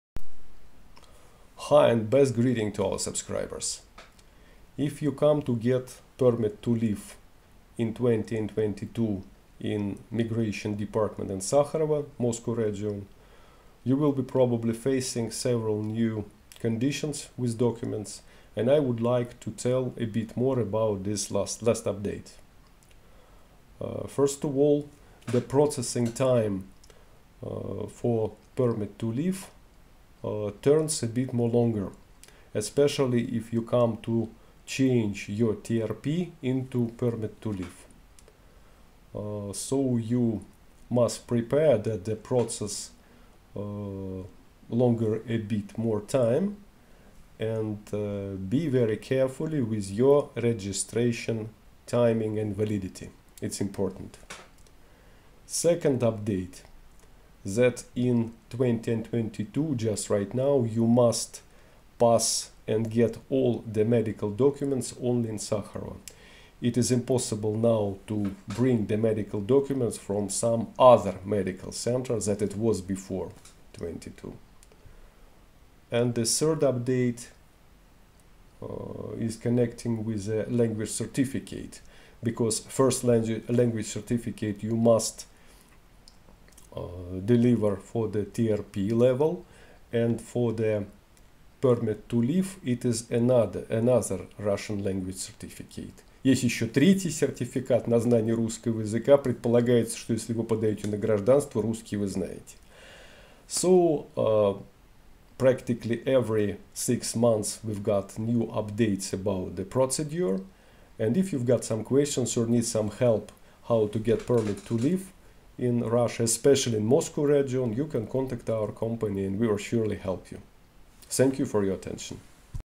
Permit to live in Russia in 2022
Last update about process how to get permit to live in Russia in 2022. New conditions of how to get medical documents, how to get russian language certificate for level VNJ and new duration of process of change TRP into permit to live. Have questions?